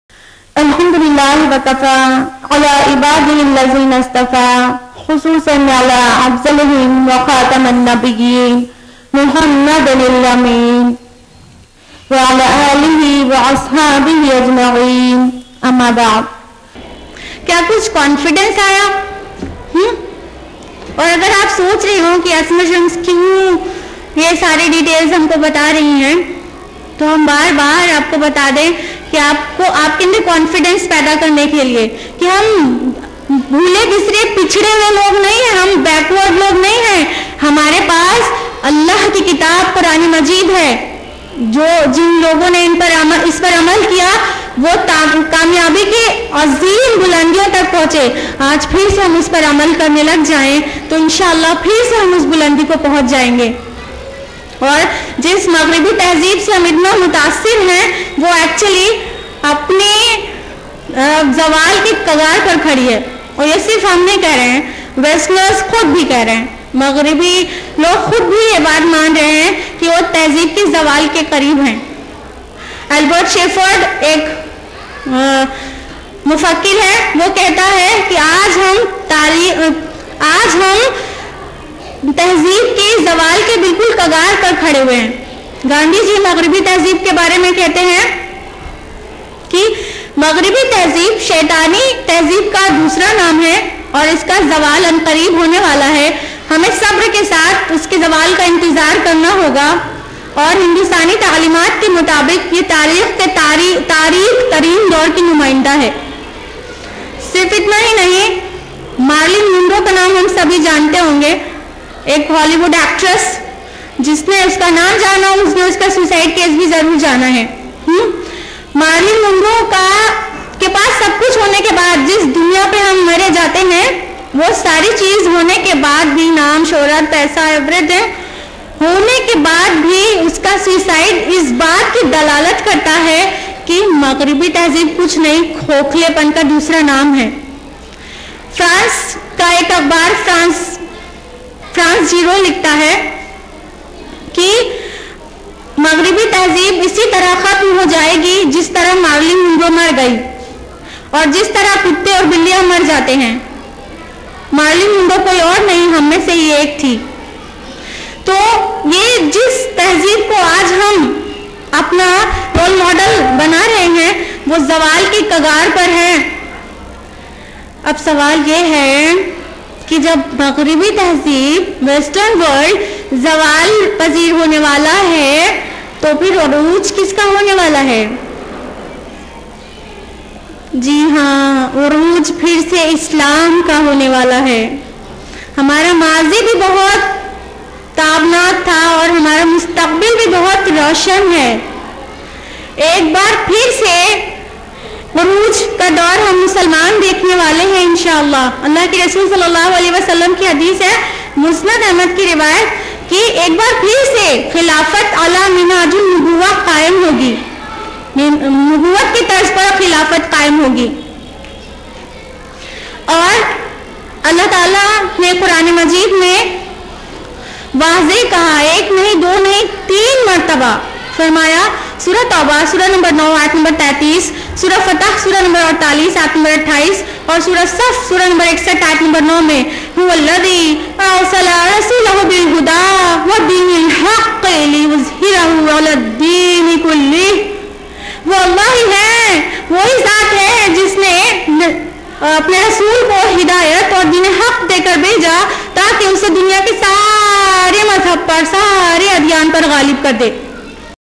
21. Western Civilization vs Islamic Civilization by Asma Shams
Millate Islamia Ke Adware Urooj Part 21